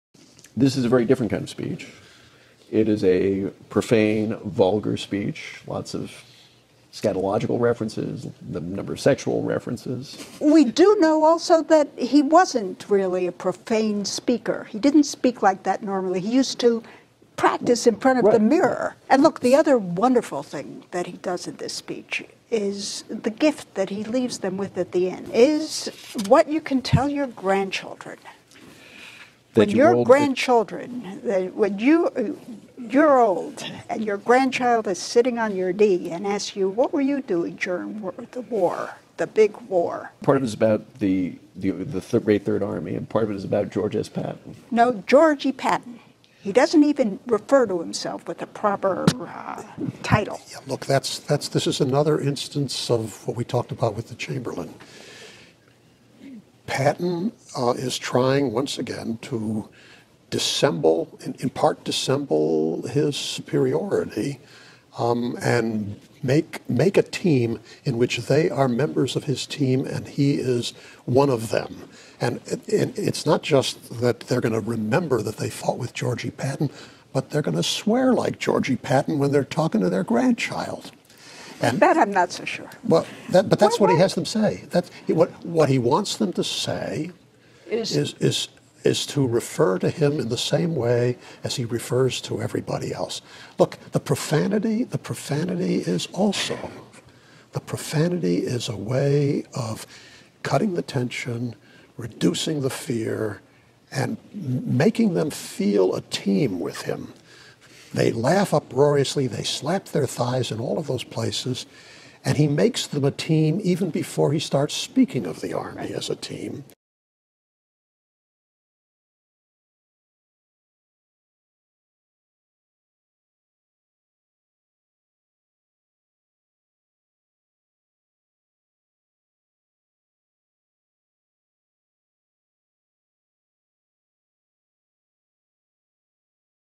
Patton: What is the function of Patton's profanity?
According to Charles M. Province, founder of The George S. Patton Jr. Historical Society and author of several books about General Patton under whom he served with great pride, George Smith Patton, Jr. (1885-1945) was a man of many—even self-contradictory—ways: "He was a noted horseman and polo player, a well-known champion swordsman, and a competent sailor and sportsman . . . an amateur poet . . . a rough and tough soldier, . . . a thoughtful and sentimental man. Unpredictable in his actions, [yet] always dependable . . . outgoing, yet introverted." Hailing from a military family that traced its lineage back well beyond the American Revolution, Patton was determined already during childhood to become a hero. After graduating from the United States Military Academy at West Point in 1909, he received a commission in the United States Army and never left it. He began as a cavalryman and swordsman, but soon became aide to General John J. Pershing, first in Mexico and then in World War I in Europe. There he became an early expert in a new form of battle machine—the tank—which he later used to full effectiveness as commander of the Third Army during World War II.

Though they often referred to him as "Old blood and guts" (a description he disliked), most of the men who served with Patton regarded him as a charismatic leader and, despite—or, according to some, because of—his copious use of profanity, an inspirational speaker. He commanded respect not only for his technical expertise, but also for his keen understanding of the human psyche (especially in wartime) and his prodigious knowledge of history and warfare. The much-celebrated movie Patton (made in 1970) makes evident his complex character, his competence, and his view of history as coherent and contiguous. It begins with the famous speech to the troops—in a much cleaned-up version.

Watch editors Amy A. Kass and Leon R. Kass converse with guest host Eliot A. Cohen (Johns Hopkins SAIS) about the story.